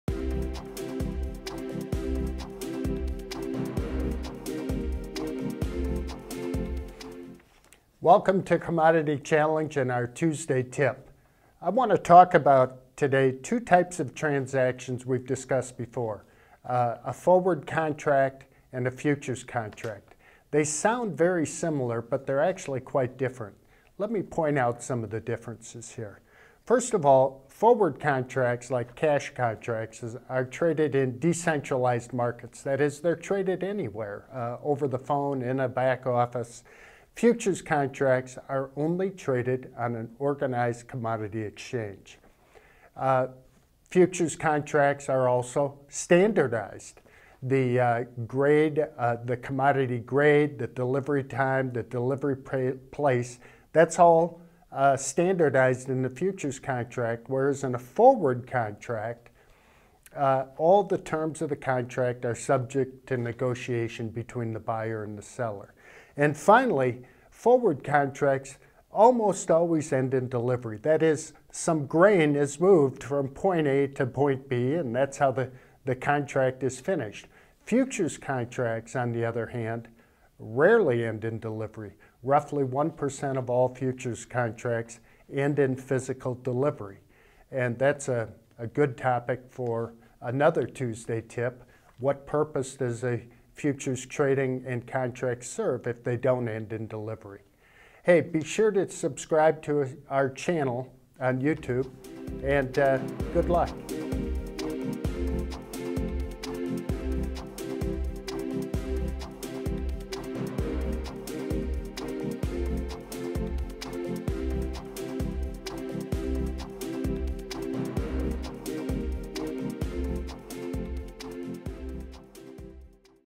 Difference Between Forward and Futures Contract - Commodity Challenge Tuesday Tip
Tuesday Tip: Many people confuse Forward and Futures contracts which sound similar but are distinctly different contracts in terms of where and how they are traded.

Futures are traded only on organized exchanges. Forward contracts are traded in decentralized markets.

Futures contracts are standardized. Forward contracts are tailored to meet the needs of a specific transaction.

Trading in futures is formalized under exchange rules that govern trading.

Futures contracts are usually not satisfied by delivery (roughly 1% of futures contracts traded end in delivery). Forward cash contracts end in delivery.